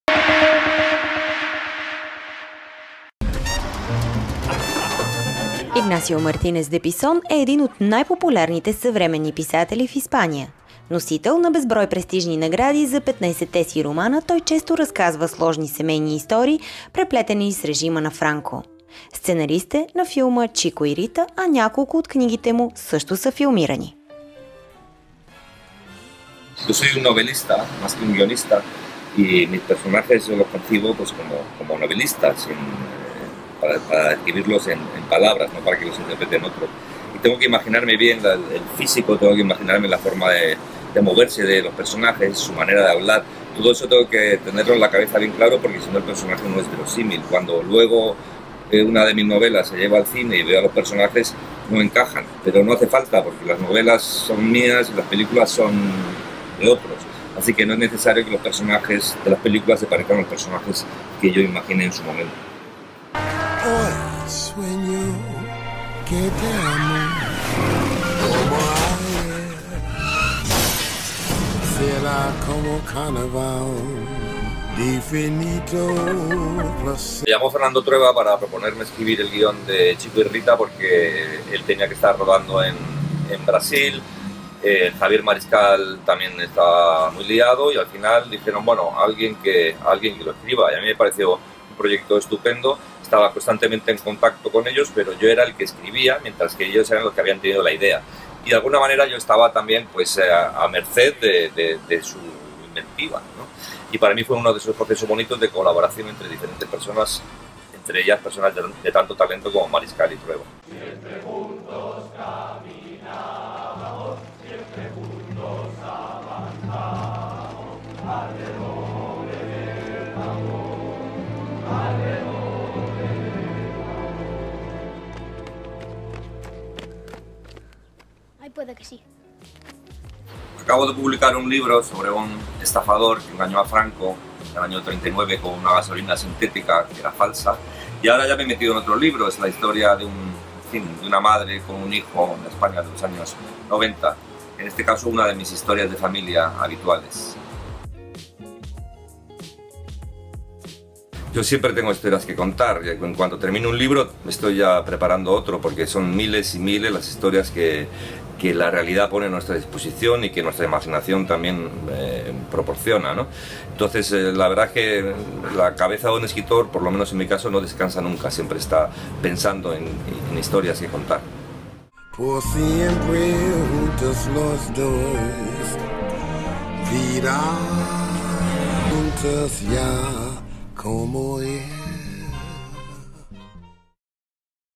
Ignacio Martinez de Pison talks about his books and characters
Spanish novelist and writer Ignacio Martinez de Pison talks about his stories, seeing his characters on the screen and writing the script for "Chico and Rita". The interview is done especially for the event Culturadas pop-up